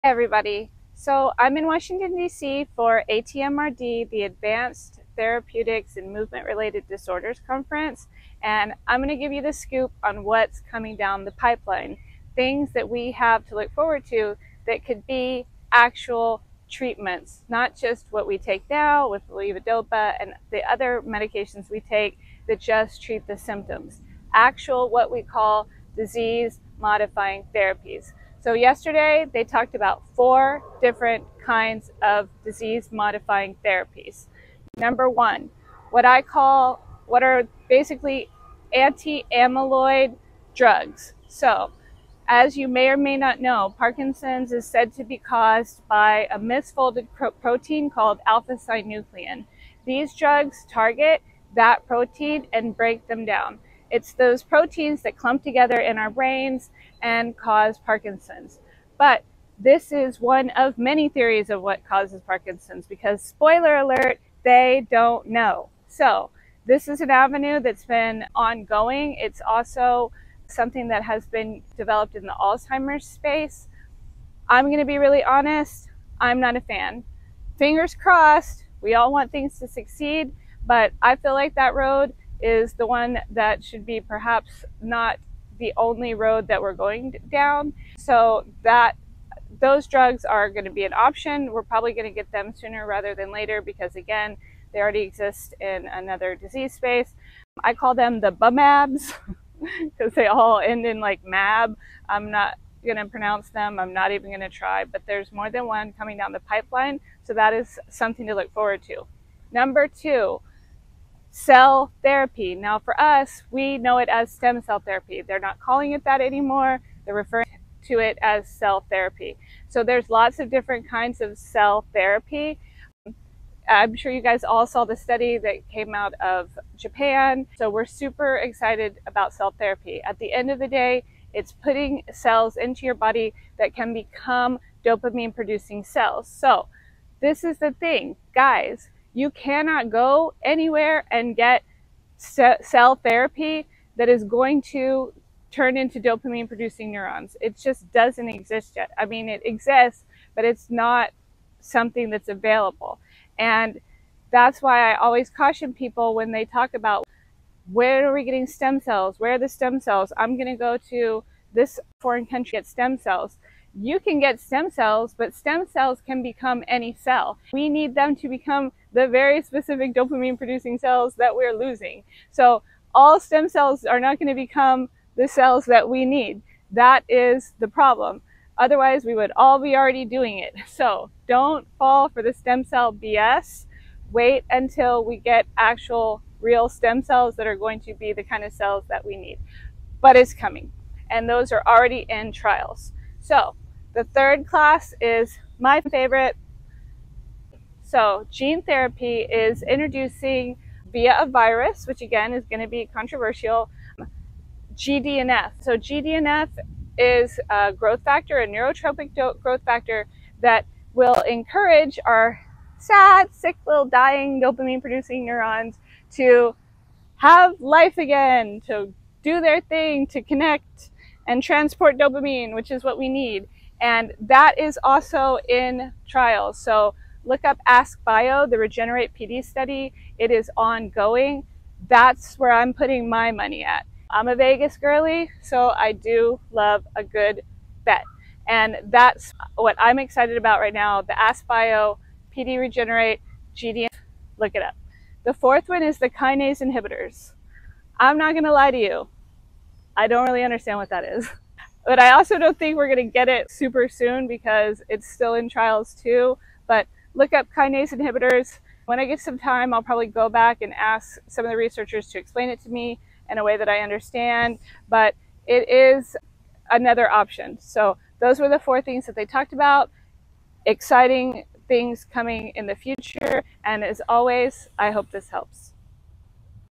ATMRD recal
Spent the weekend with neurologist and learning about research